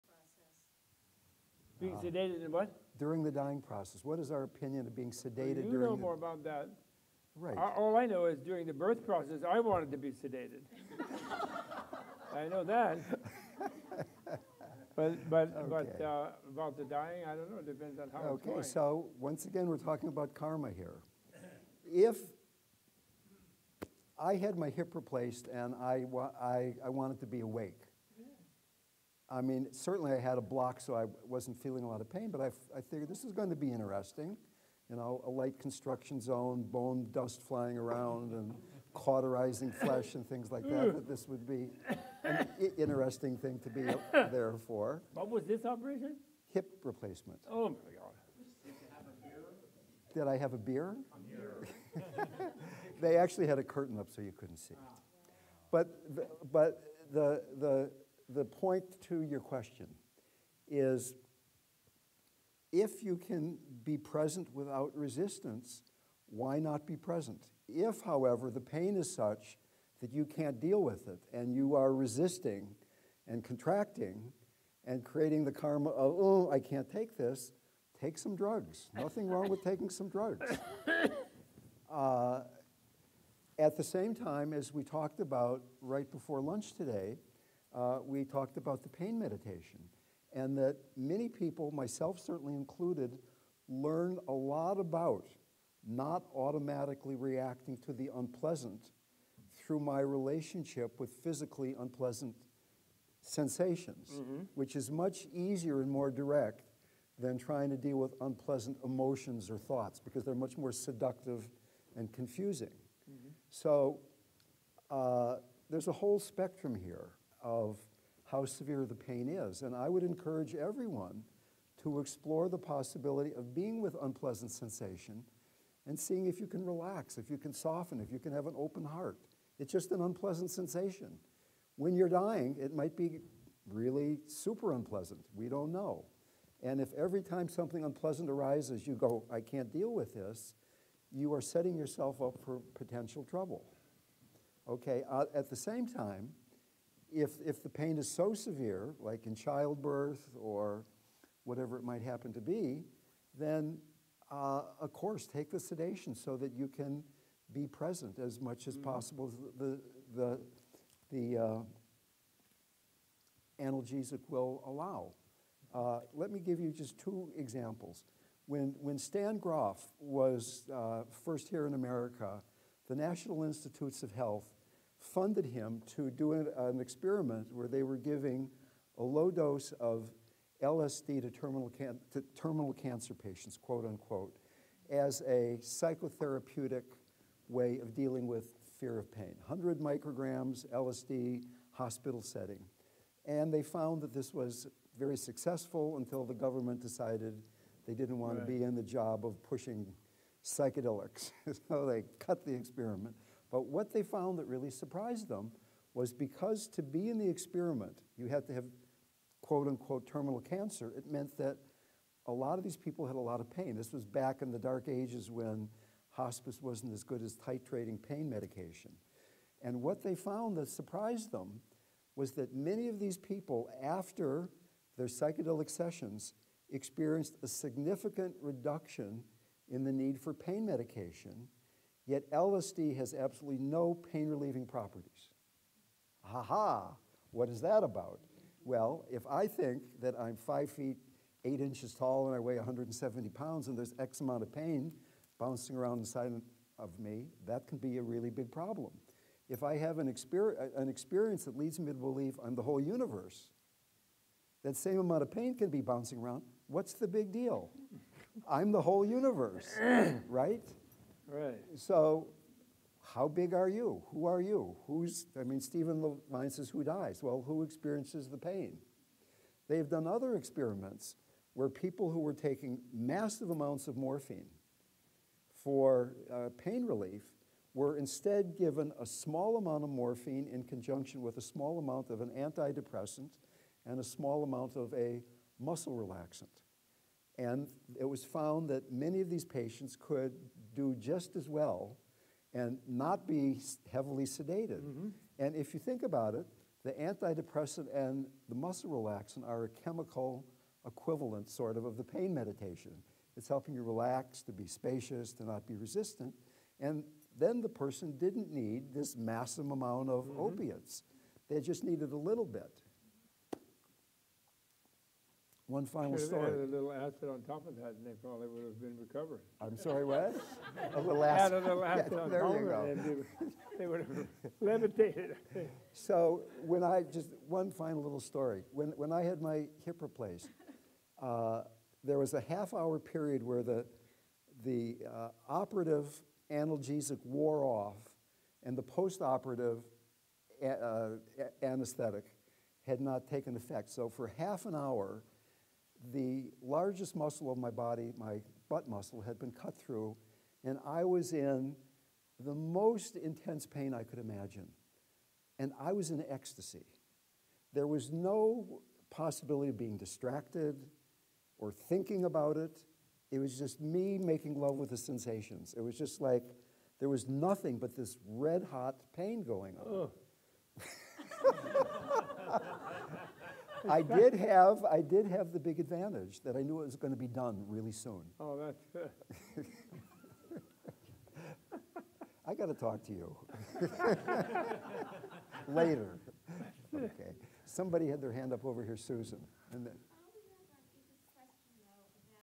How are Karma, LSD, Antidepressants & Pain Medication Related? Dale Borglum & Robert Thurman
In this video Robert Thurman & Dale Borglum discuss the Buddhist perspective on the use of medications + their relationship with karma.

Recorded at the 2015 Death & Deathlessness: Buddhist Insights and Practices for Life, Death & the In-Between Retreat Dale Borglum & Robert Thurman.

Halloween Evening Discussion.

Recorded at Menla's Nalanda Conference Center in Phoenicia, New York during the Oct 31st 2015 Halloween Evening Discussion of the 2015 "Death & Deathlessness: Buddhist Insights and Practices for Life, Death & the In-Between Retreat" with Dale Borglum & Robert Thurman.

Full Access begins at $2 a month. Connect with Tibet House US + Menla Online:

🗽 Tibet House US | New York City

🏞 Menla Retreat | Phoenicia, New York